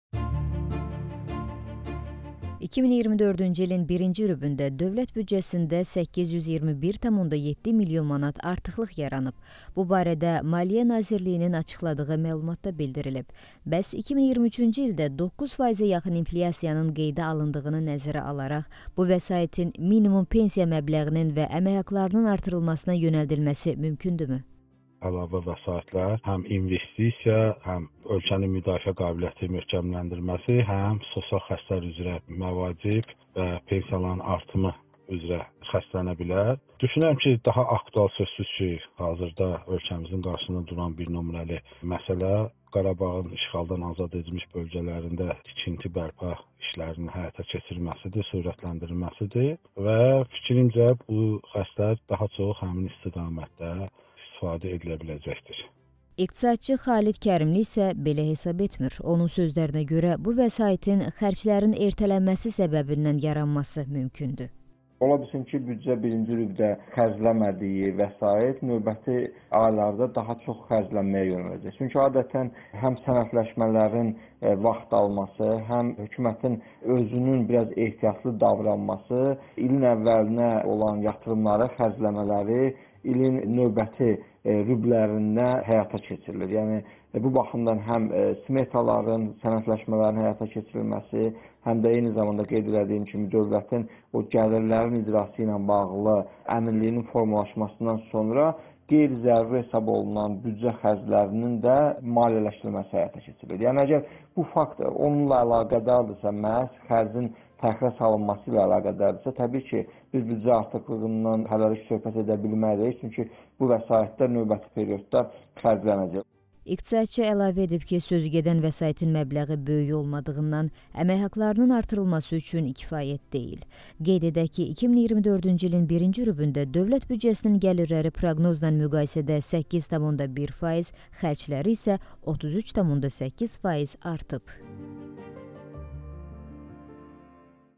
Büdcədə nə qədər artım yaranıb? | İqtisadçıdan açıqlama
2024-cü ilin birinci rübündə dövlət büdcəsində 821,7 milyon manat artıqlıq yaranıb.
Bu barədə Maliyyə Nazirliyinin açıqladığı məlumatda bildirilib.
Bəs 2023-cü ildə 9 faizə yaxın inflyasiyanın qeydə alındığını nəzərə alaraq, bu vəsaitin minimum pensiya məbləğinin və əməkhaqlarının artırılmasına yönəldilməsi mümkündürmü?